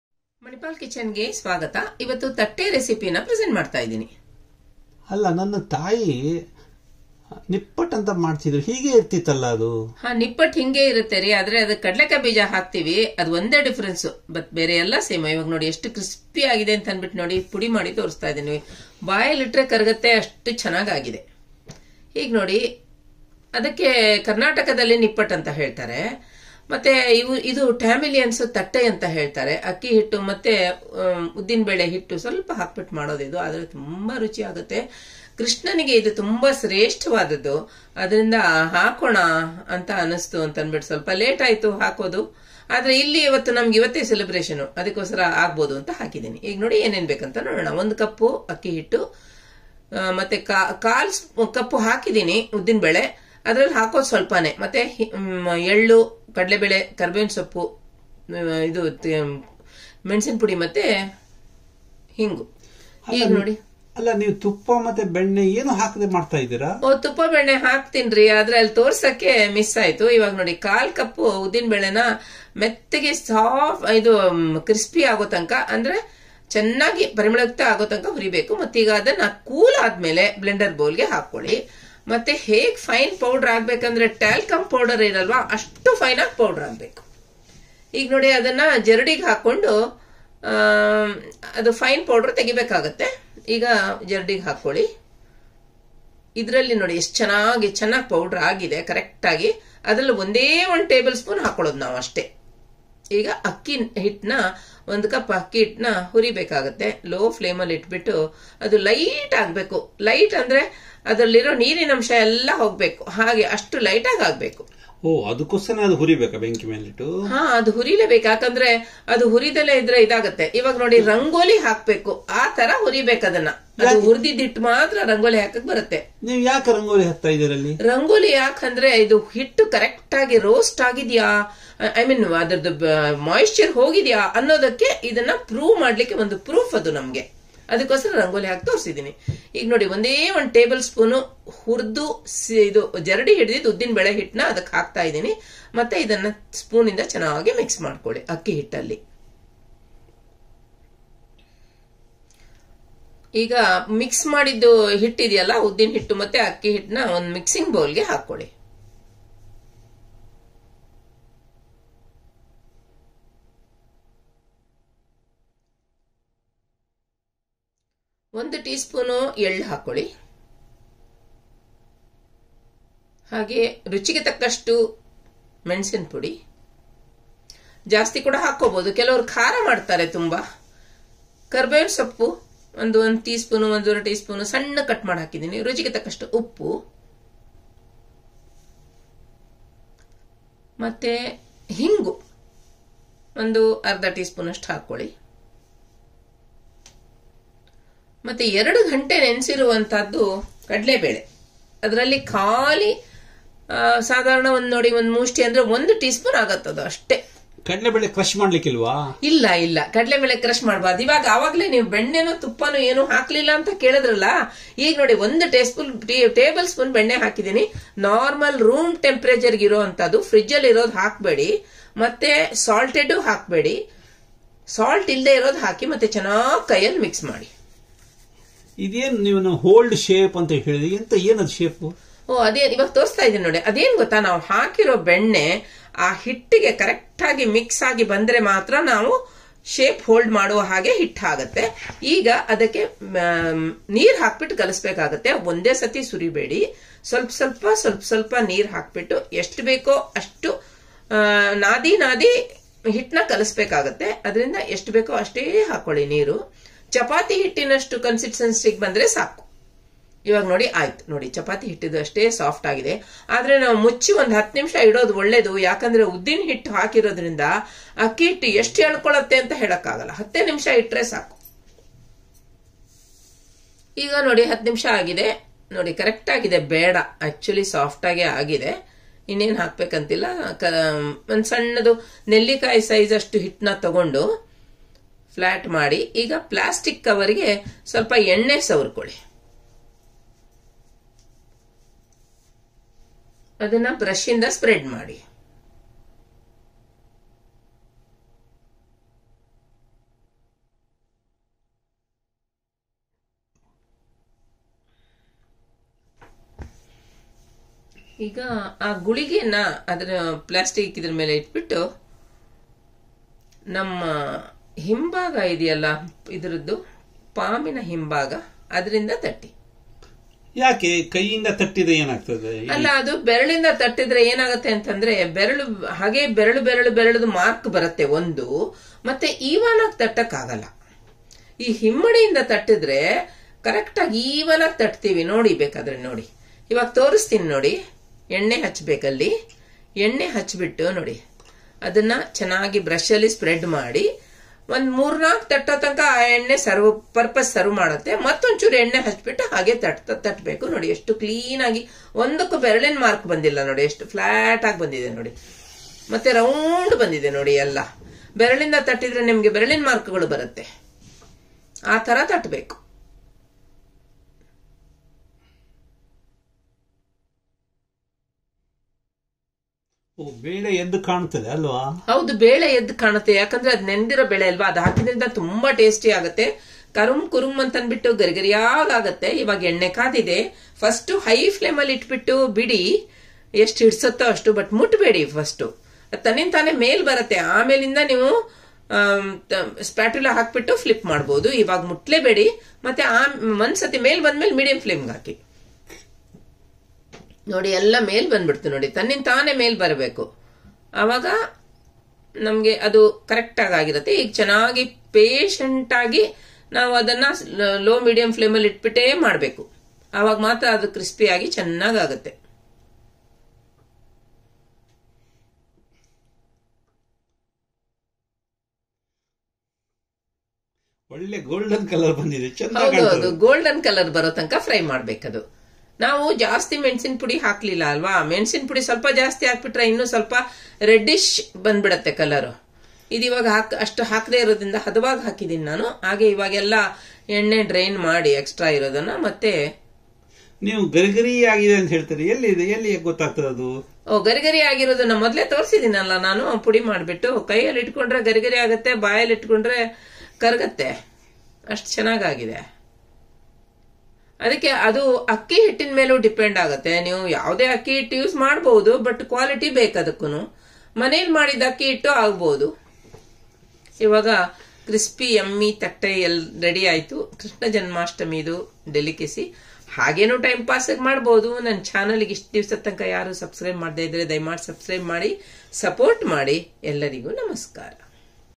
1ತಿಂಗಳು ಕೆಡಲ್ಲ,ಸೋಡಾ ಇಲ್ಲದೆ,ಗರಿಗರಿ ಬಾಯಲ್ಲಿಟ್ಟರೆ ಕರಗುವ ತಟ್ಟೈ,| THATTAI ,No SODA ,CRISPY FOR 1 MONTH,
ಸೋಡಾ ಇಲ್ಲದೆ, ಗರಿಗರಿ ಬಾಯಲ್ಲಿಟ್ಟರೆ ಕರಗುವ ತಟ್ಟೈ,ಜನ್ಮಾಷ್ಟಮಿ ಪ್ರಸಾದ| Crispy, Thattai, Janmashtami Delicacy |English Sub-Titles| Manipal Kitchen
Thattai in tamil nadu as it is called is Nippattu in Karnataka.  The content being the same except that karnataka people add broken peanut pieces to it as an additional ingredient.  People use the chapati presser to make the thattai , but the traditionl method is the way I have shown it in the video here. Here goes the recipe for Thattai.
INGREDIENTS :
1. Rice Flour/ atta - 1 cup
2. Roasted Urad dal flour - 1 tbsp
3. Hing powder - 1/2 tsp
4.  Chopped curry leaves - 1 tsp
5. Soaked chana dal for 2 hours - 1 tsp
6. White sesame seeds - 1 tsp
7. Red chili powder - as per taste
8. Unsalted butter - 1 tbsp
9. Salt - to taste
10. Water - as it holds
PROCEDURE: Dry roast rice flour on low-medium flame till it is light.  The proof to know if light is to draw lines as shown in the video.  Dry roast urad dal on low flame till aromatic and crispy but not to roast too much as it is fried in oil later on.  Dry roast as shown in the video.  On cooling, transfer roasted urad dal to a blender and powder it to the consistency of talcum powder.  Rice flour also has to be fine like talcum powder.  But on roasting it will be fine.  Sieve urad dal flour and add it to the roasted rice flour and mix till combined well.  Transfer the mixed flour to a mixing bowl and add the remain ingredients  the like hing powder, chopped curry leaves, soaked chana dal. white sesame seeds, red chili powder, and mix till combined well with the spoon.  Add unsalted butter and rub it to the atta mix so that the mix holds shape.  Add water in steps and bring it to the consistency of chapati dough and rest for 10 minutes.  Drizzle oil and spread it with the help of a brush all over the plastic sheet and place the pinched out dough and shape it like a small papad with the help of palm by applying oil to it and  repeat the same process with the remaining dough.  Place a skillet with 2 cups of oil and heat on high flame. Slide thattai in it and in the begining  on high flame and then fry on medium flame till crispy and when done, transfer to a colander and enjoy and comment feedback on the channel.
Makes: 25-30 of the above size